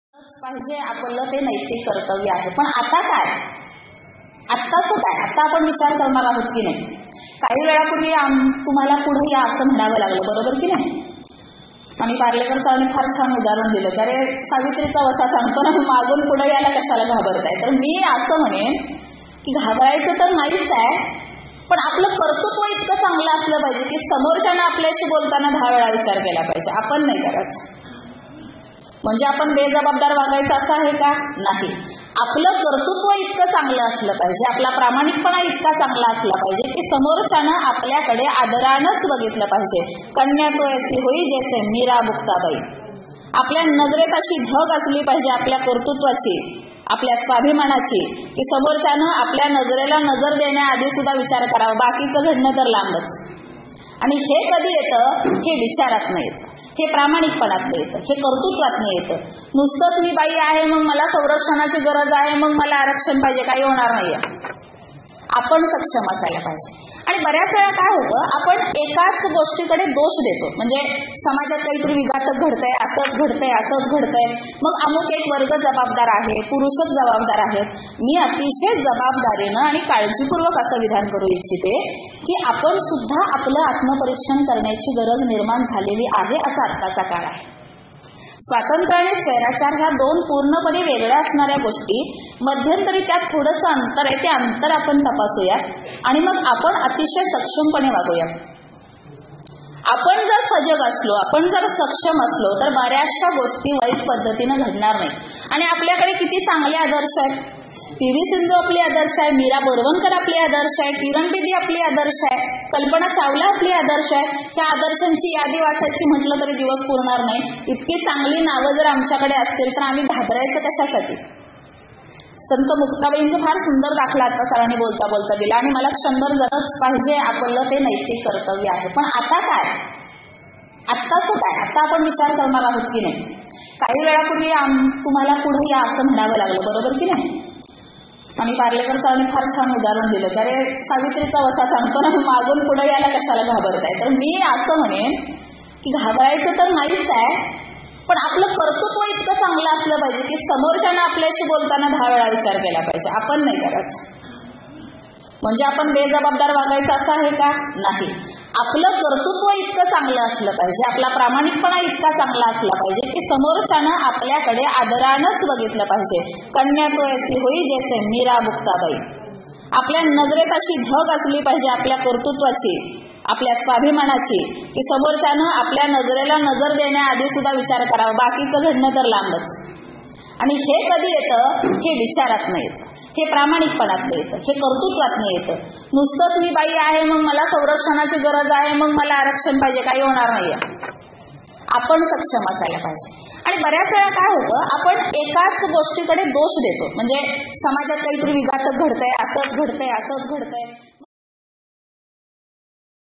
लता ऐवळे - कदम महिला दिन भाषण
महिला दिनानिमित्त झालेले लता ऐवळे - कदम (Lata Aivale - kadam यांचे भाषण